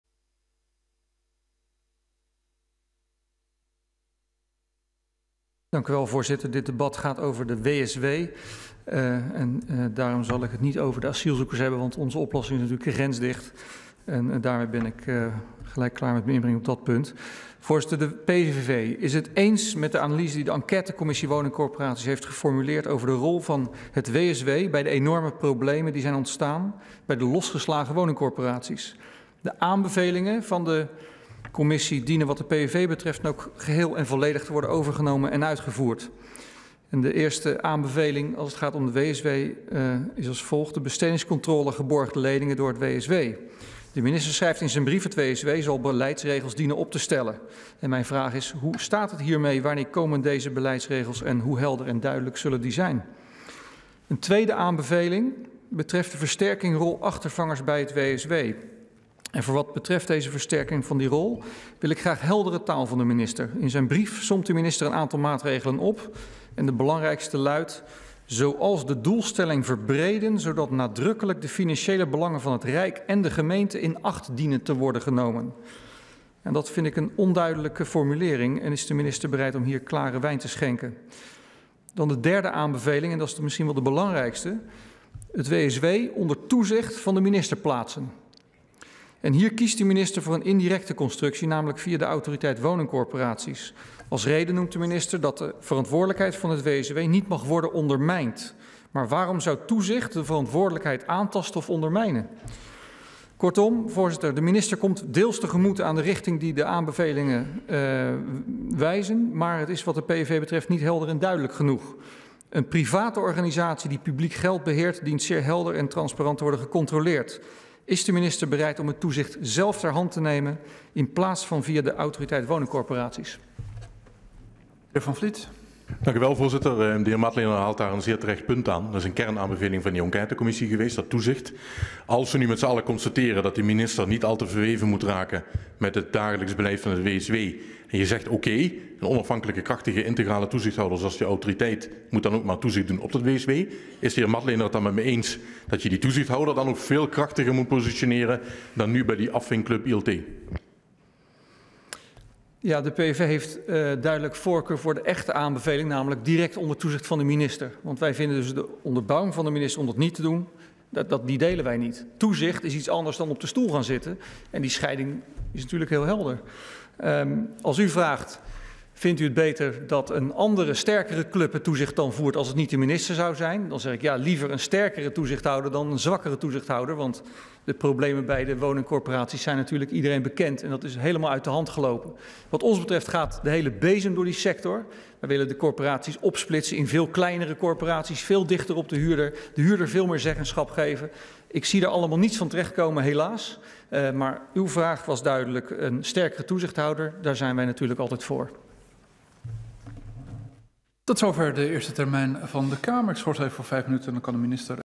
Barry Madlener - Debat op hoofdlijnen over de financiële kaders van het WSW (2015)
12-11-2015

Barry Madlener - Debat op hoofdlijnen over de financiële kaders van het WSW